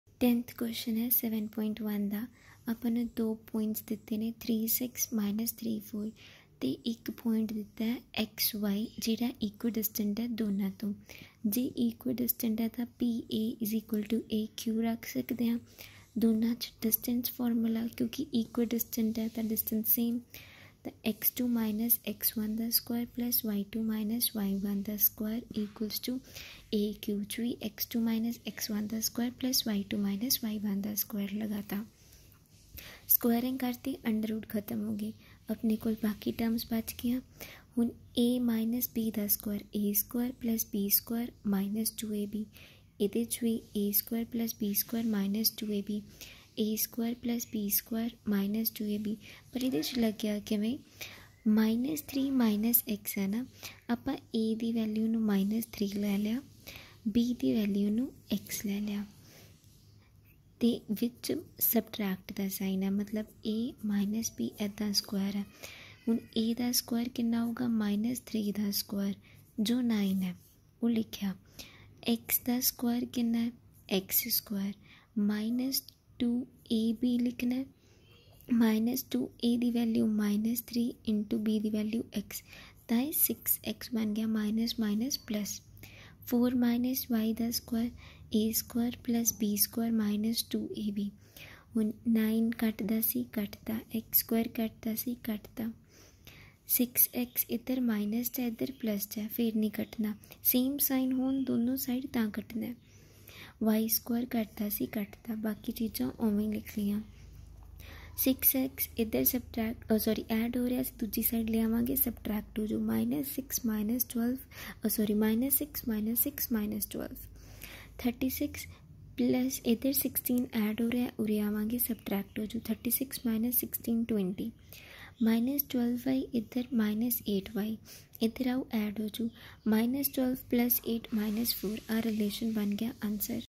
Maths ncert class10 chapter7 ex 7.1 question10 | coordinate geometry
Find a relation between x and y such that the point (x, y) is equidistant from the points (3, 6) and (-3, 4)